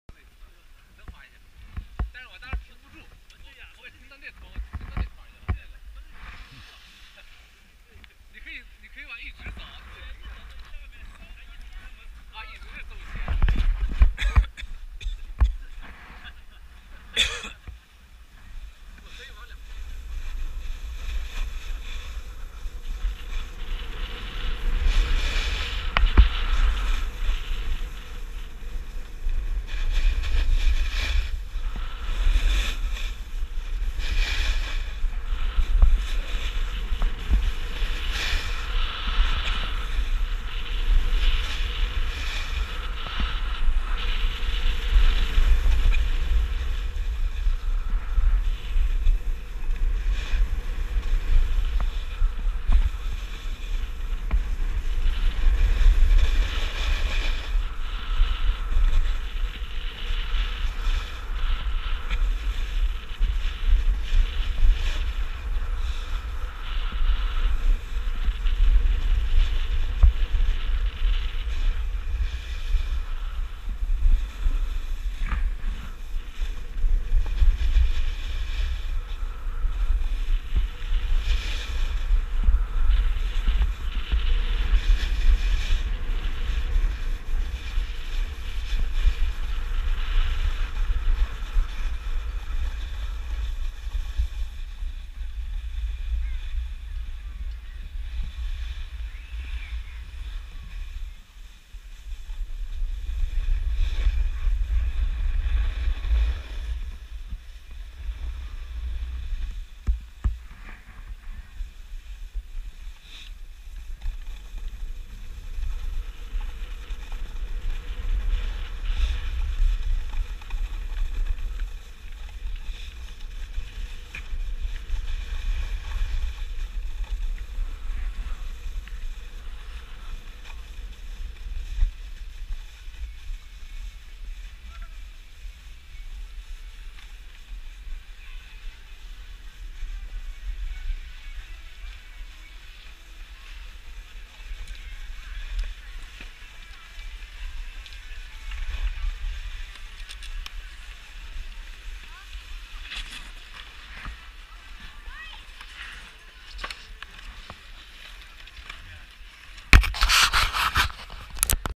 2014-12-21 Mike B Kop snowboarding with GoPro on Helmet Mount
From Clyde Super Slide to the Ski Lodge